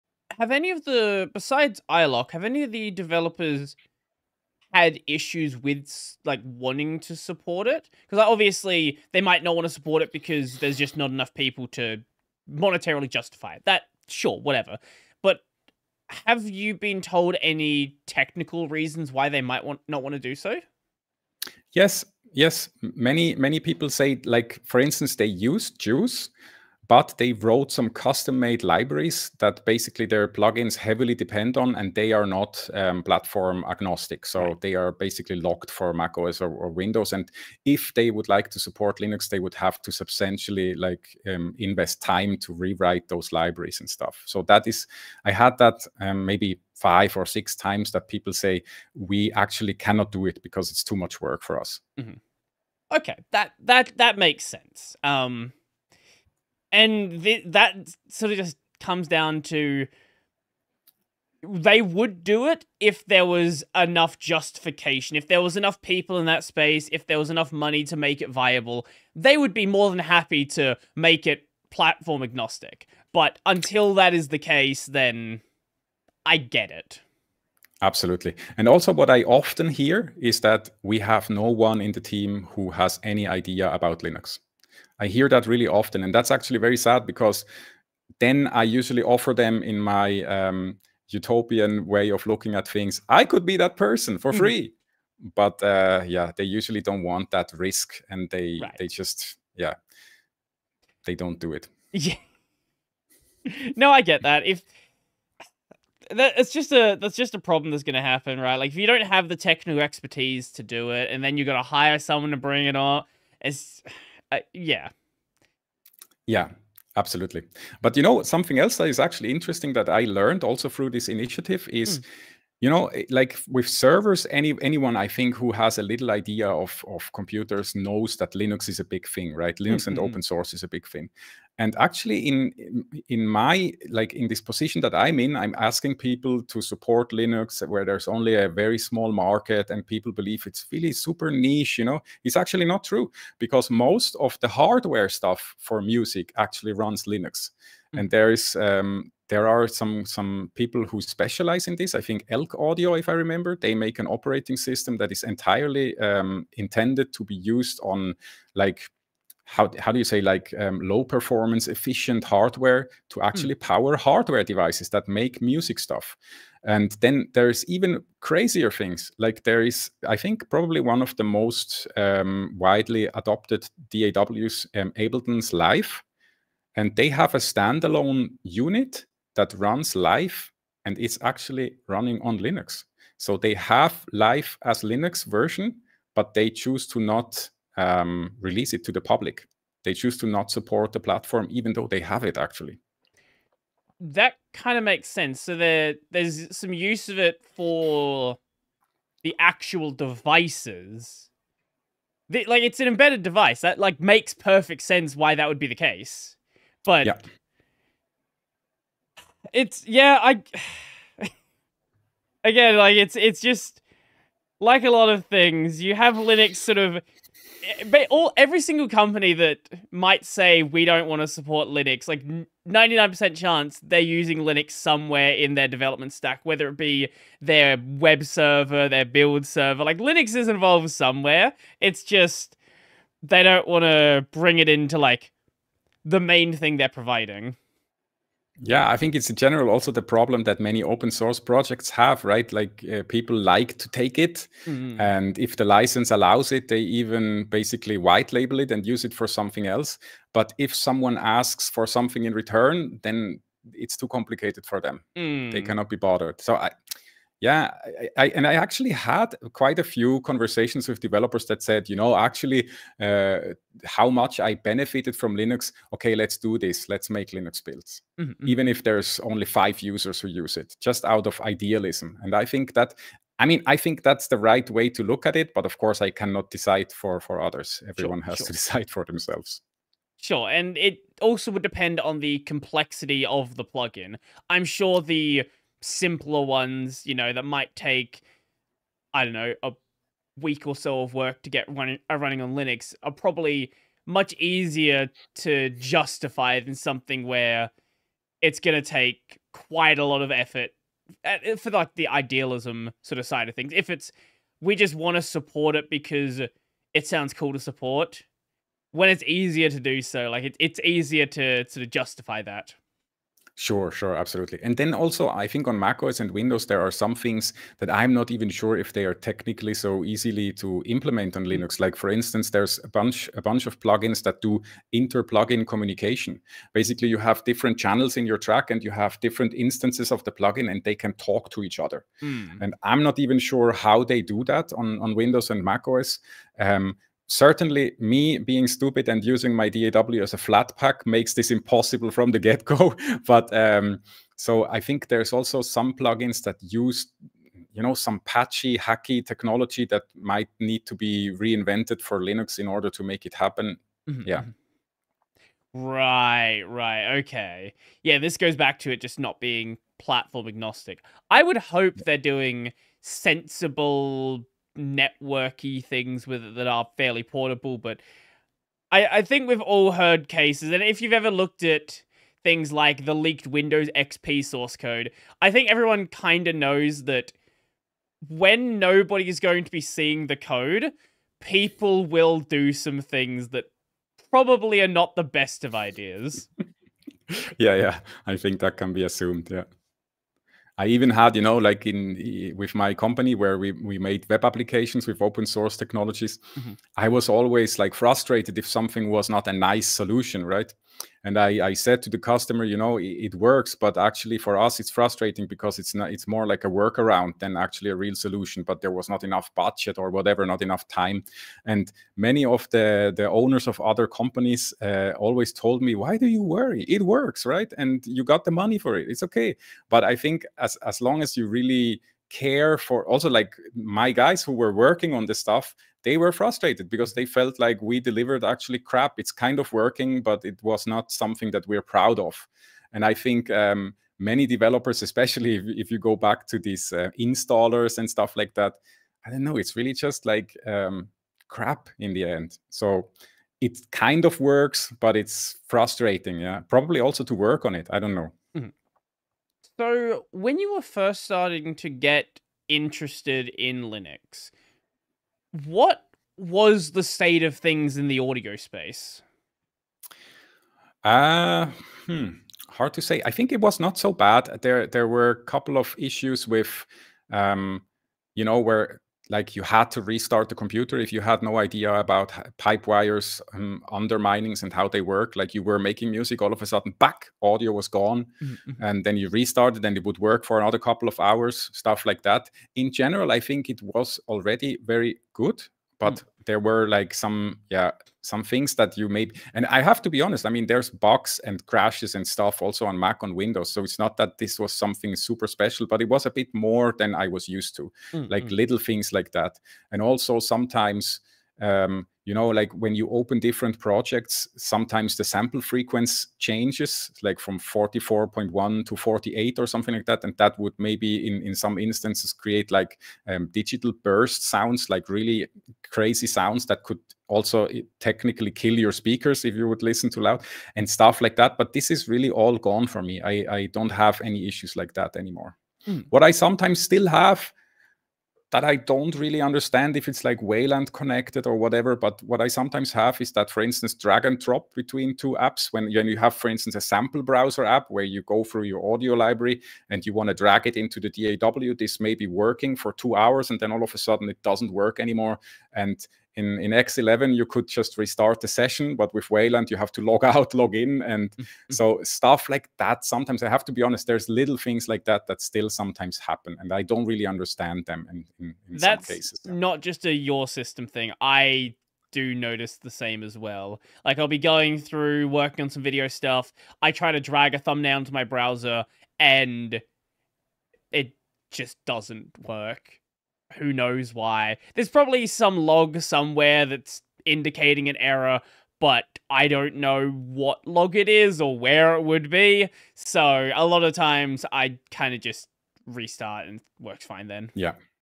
Why Don't Audio Plugin Devs Just Support Linux
Why don't more audio plugins just support Linux, it's not like there's none but there are these quite large gaps in the market.